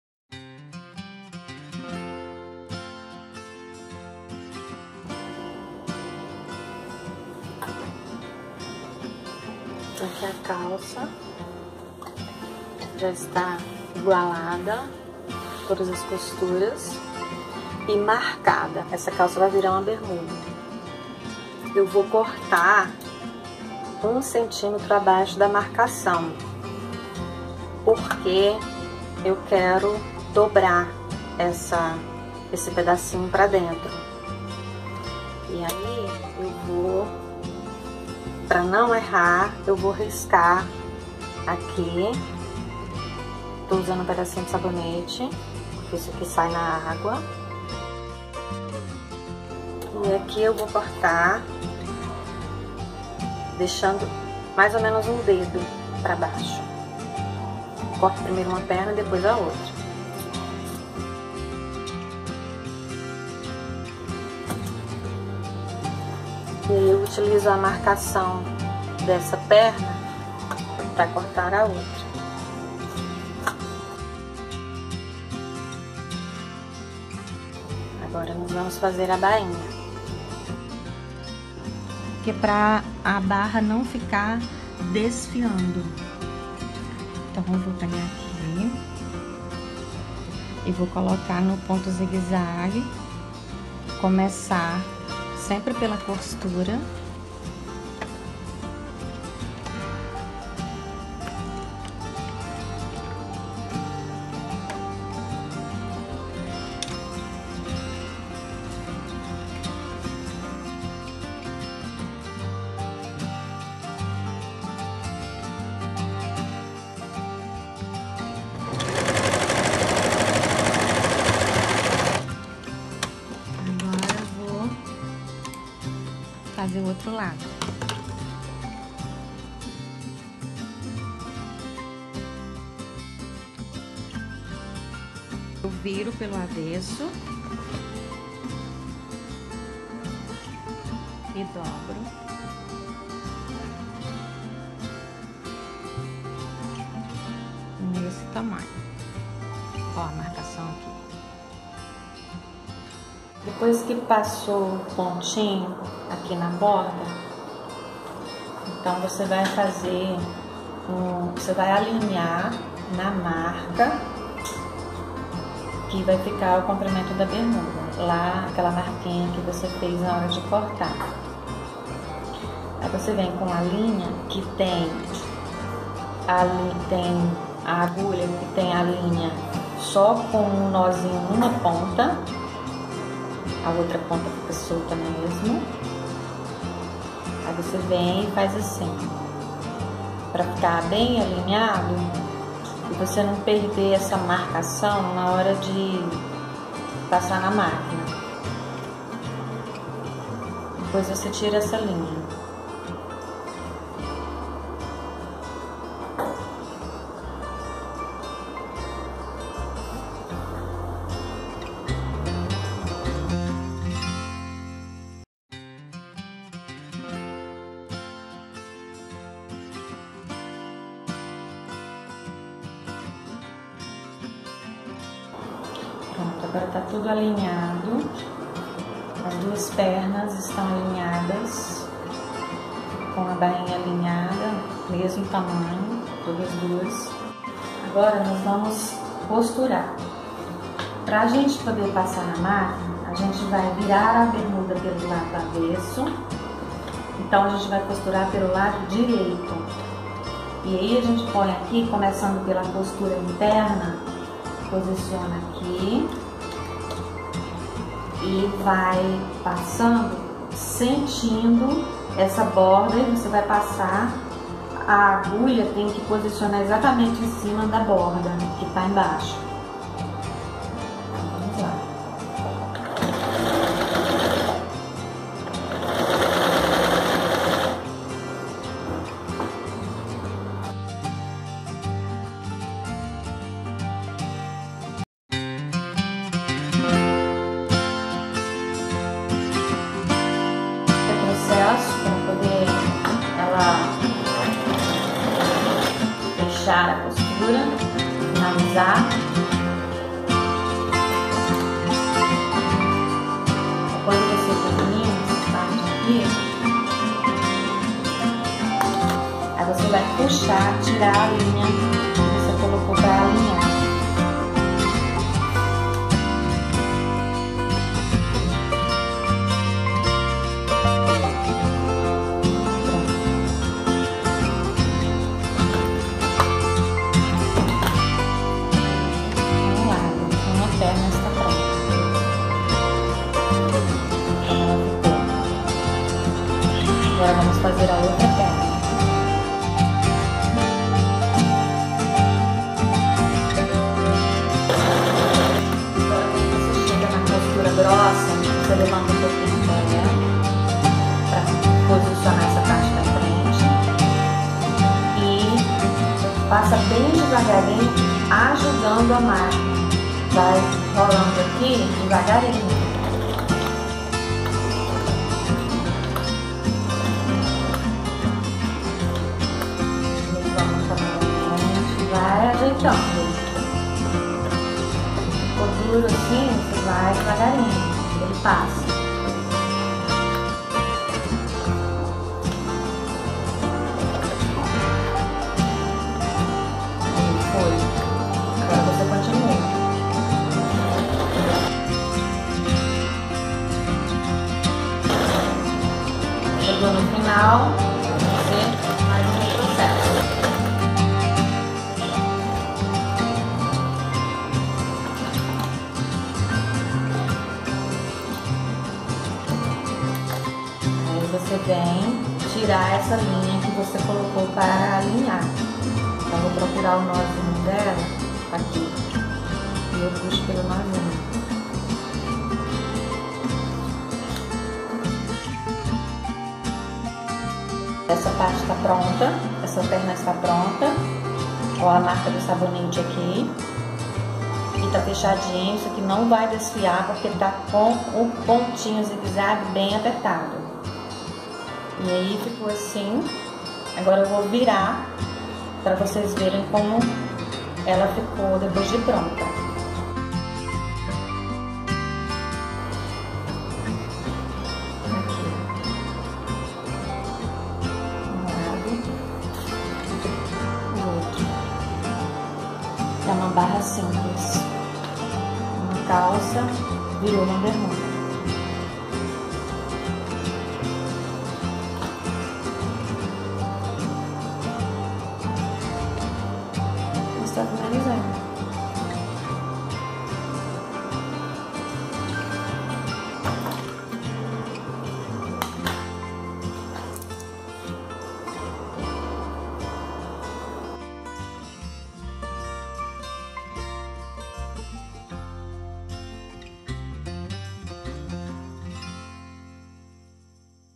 DIY Como cortar calça jeans para fazer Bermuda | Mia Dicas
DIY Como cortar calça jeans para usar como bermuda e fazer uma barra simples utilizando a máquina de costura doméstica. Esse mesmo tipo de barra também pode ser feito na calça. É uma barra bem simples e fácil de fazer. Virei a calça pelo lado avesso para passar a costura, mas também pode fazer pelo lado direito. Não tem problema, fica até mais fácil.

Tem uma edição desse vídeo ensinando a fazer de outra maneira e sem alinhavar, apenas usando alfinetes.

Se você gostou do vídeo, clique nos botões curtir e inscrever-se :)

Para ler todas receitas acompanhe o blog ou o site. Lá estão todas as receitas, inclusive as que ainda não foram filmadas.